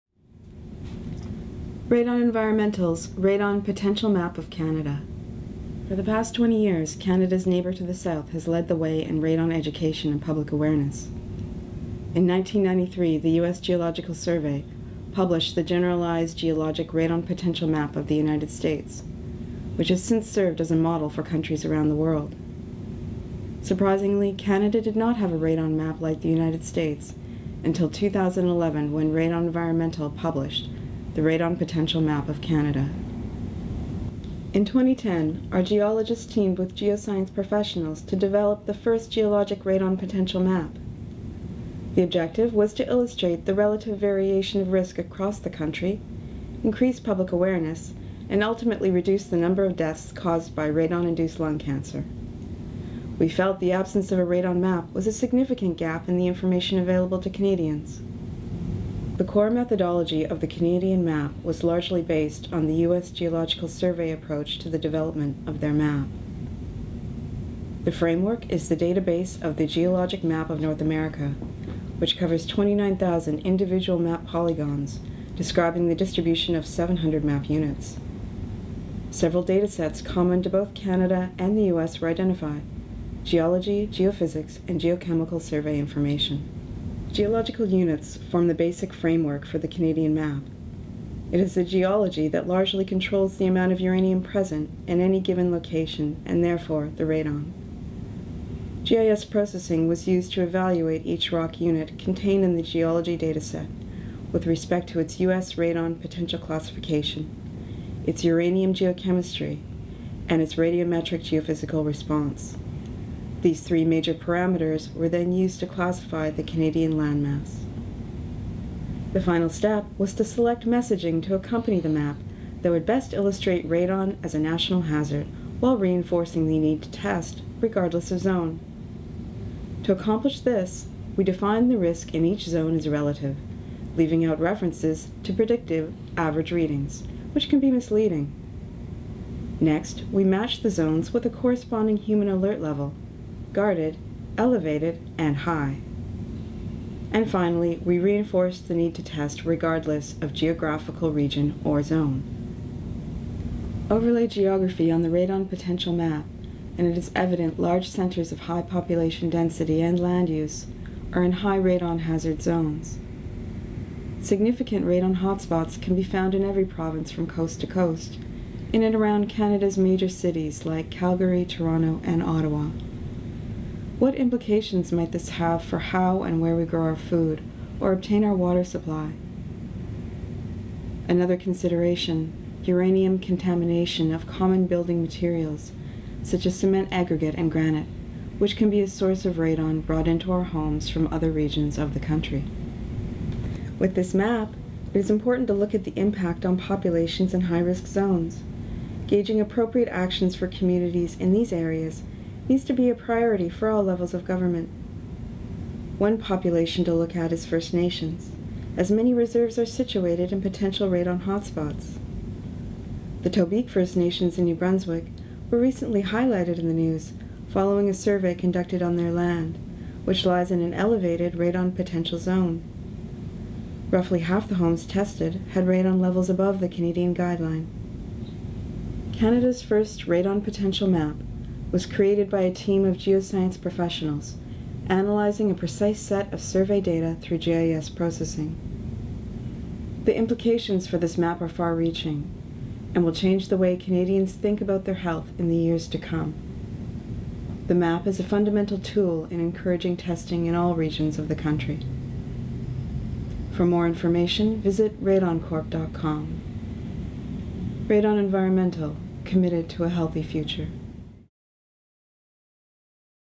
Radon Environmental Unveils NEW Radon Potential Map of Canada - 2011
Radon Environmental Management Corp. has recently unveiled a new Radon Potential Map of Canada.